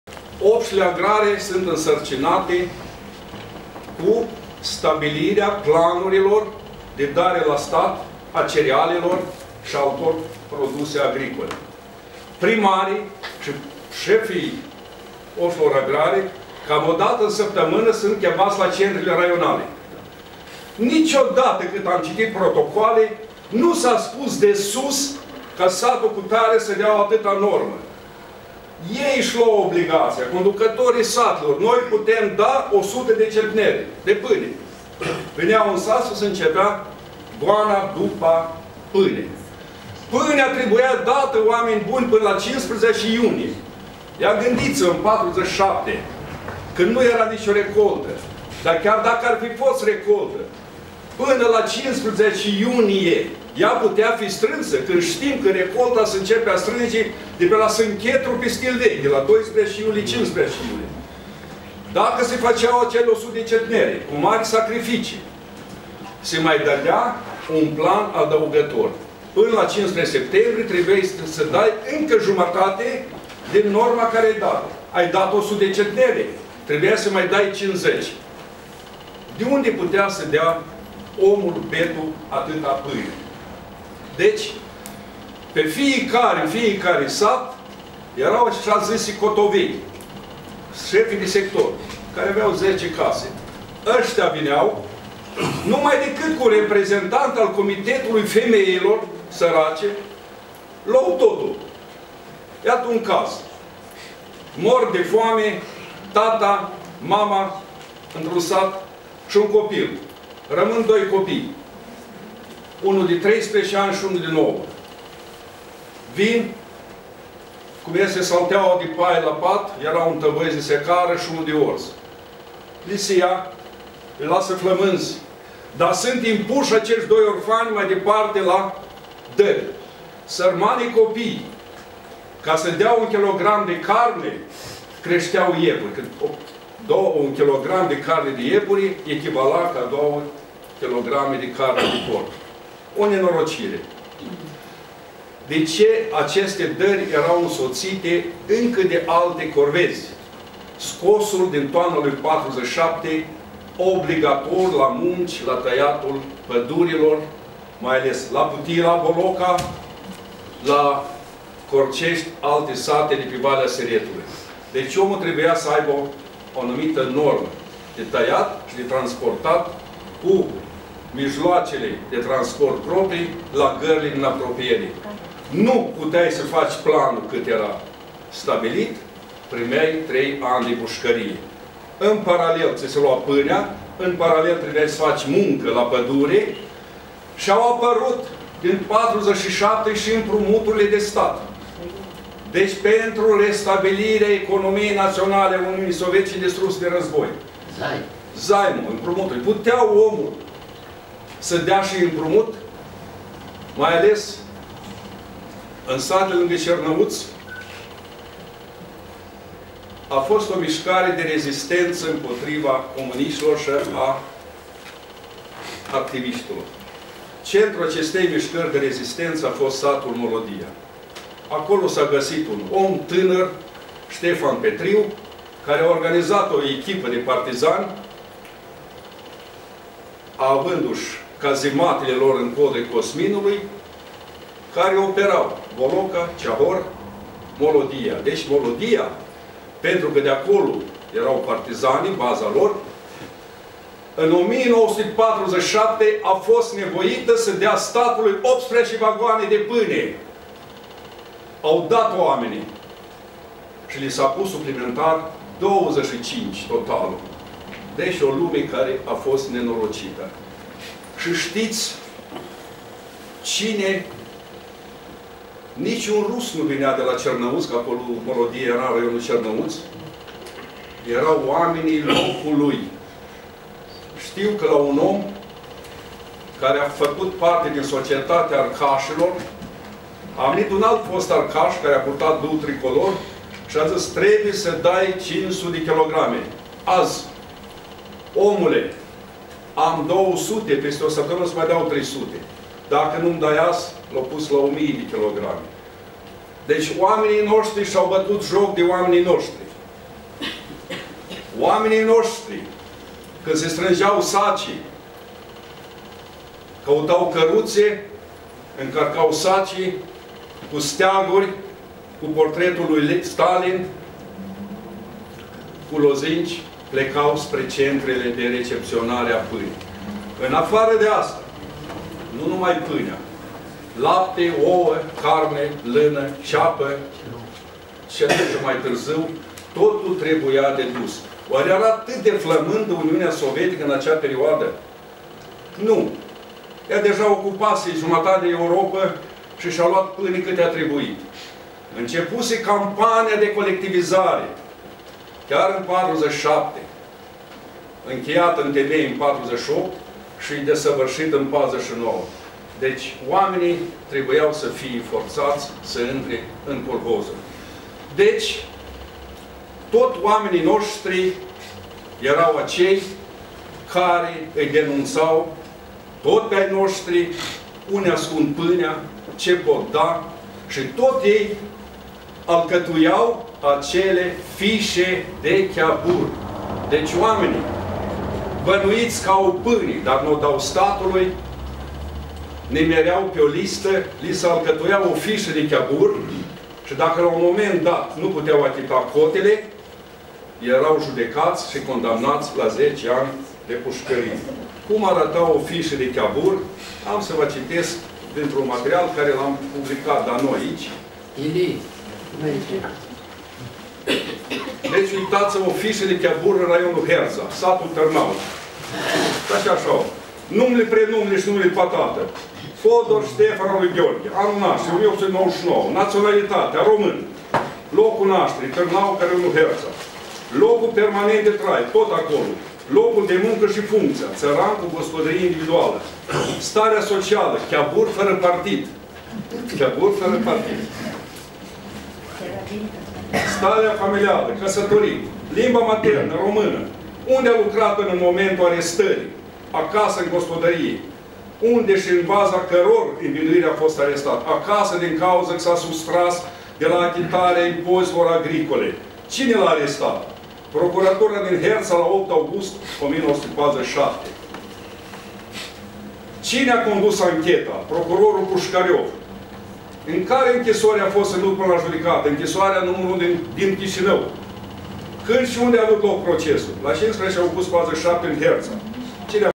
CONFERINȚA INTERNAȚIONALĂ «FOAMETEA ÎN UCRAINA (1932 - 1933) ŞI (1946 - 1947)»
CONFERINȚA  INTERNAȚIONALĂ
«FOAMETEA  ORGANIZATĂ DE REGIMUL  TOTALITAR  ÎN  UCRAINA (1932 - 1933)
ŞI  NORDUL  BUCOVINEI (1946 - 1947)»